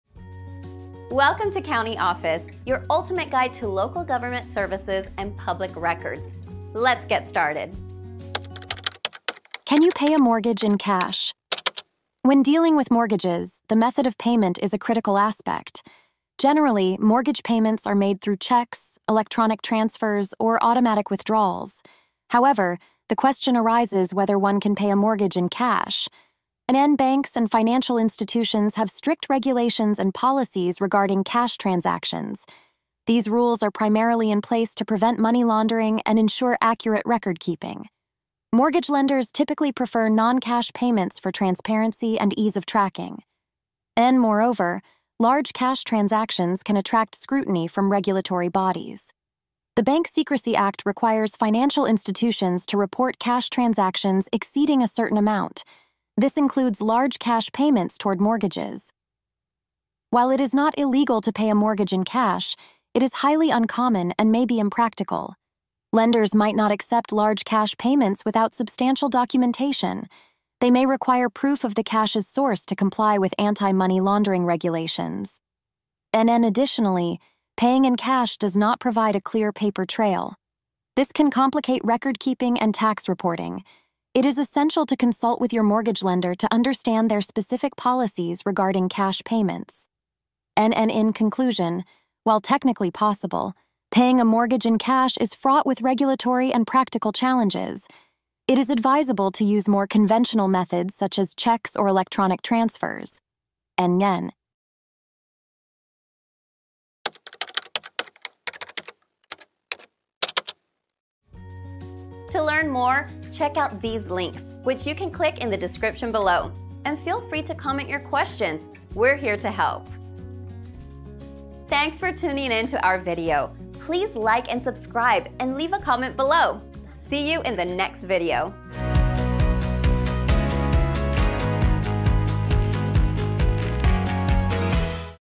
Can You Pay A Mortgage In Cash? - CountyOffice.org
Can You Pay A Mortgage In Cash? Have you ever considered the possibility of paying your mortgage in cash? In this enlightening video, we delve into the intricacies of cash payments for mortgages. Discover the regulations surrounding cash transactions with banks and financial institutions, and why paying a mortgage in cash may not be the most practical option. Learn about the implications of large cash transactions on mortgage payments and the importance of maintaining a clear paper trail for record-keeping and tax reporting.

Consult with your mortgage lender to understand their policies on cash payments and explore alternative payment methods such as checks or electronic transfers for a smoother transaction process. Stay informed on the regulatory and practical challenges associated with paying a mortgage in cash to make the best decision for your financial situation.